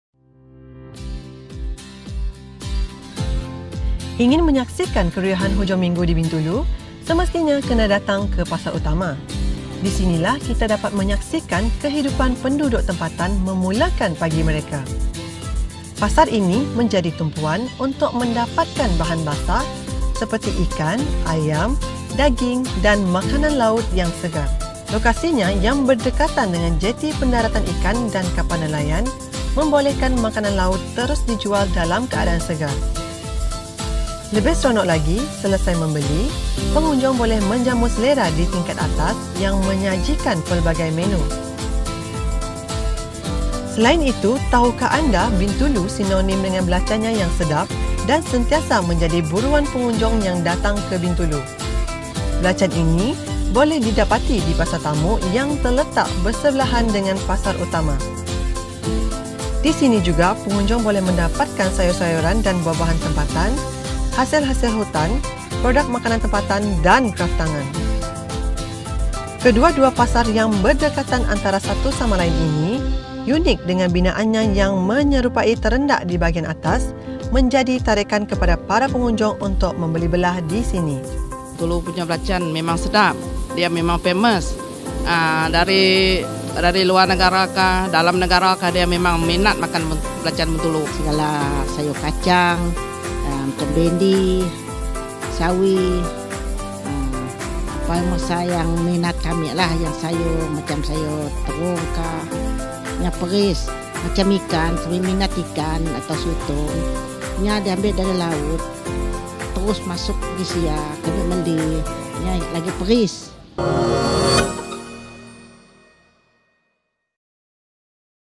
PASAR UTAMA DAN TAMU BINTULU
Ingin makanan laut yang segar? Anda
wajib singgah di Pasar Utama Bintulu. Lokasinya yang
berdekatan dengan jeti pendaratan ikan dan kapal
nelayan membolehkan makanan laut dijual dalam
keadaan segar.

Selain itu, Bintulu juga terkenal dengan belacannya
yang sedap dan menjadi buruan pengunjung. Belacan
ini boleh didapati secara terus di Tamu Bintulu yang
hanya terletak bersebelahan dengan Pasar Utama.